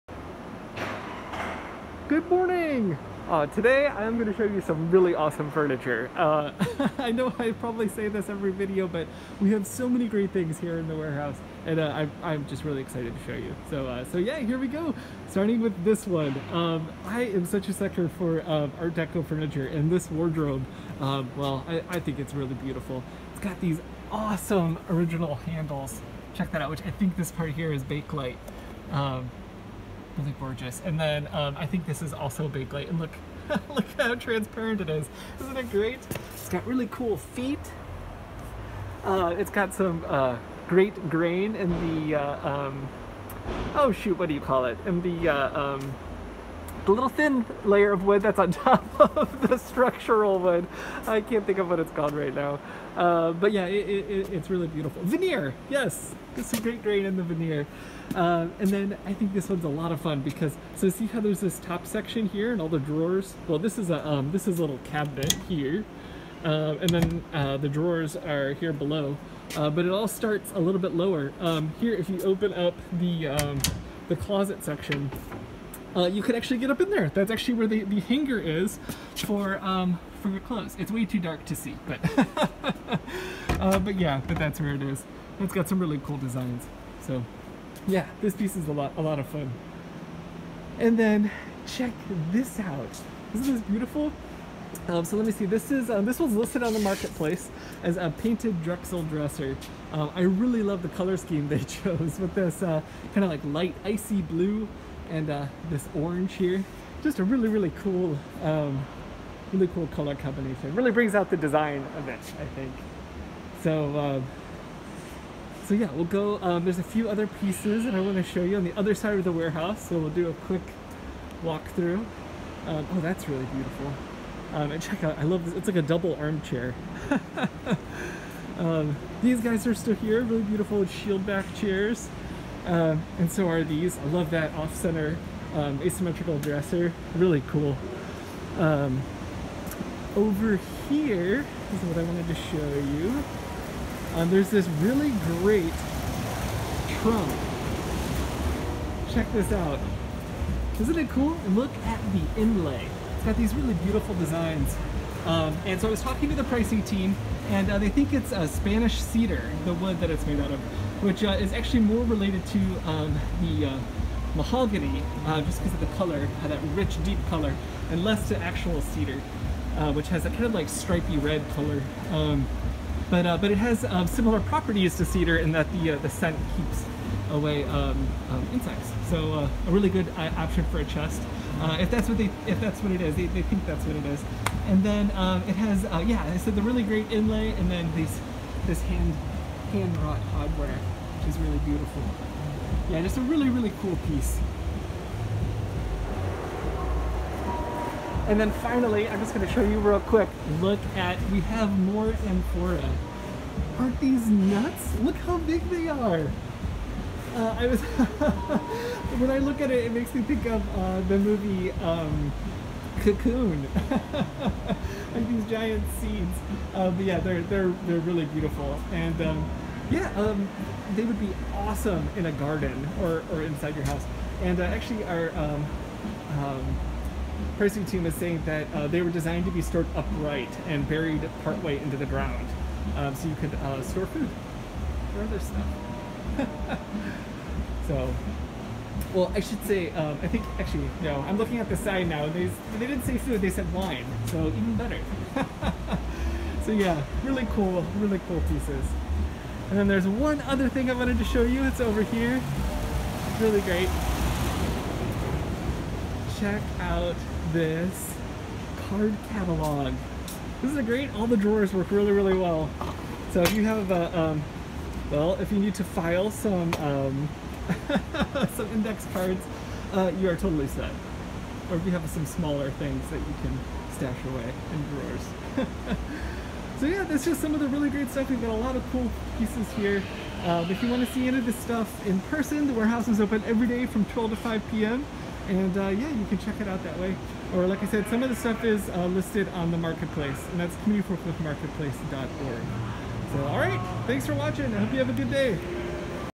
Antique chest, wardrobe, card catalogue, and more!
A beautiful antique inlaid chest, awesome Art Deco wardrobe, large Mediterranean amphorae, library card catalogue, and more. There are a lot of great items in the reuse warehouse right now!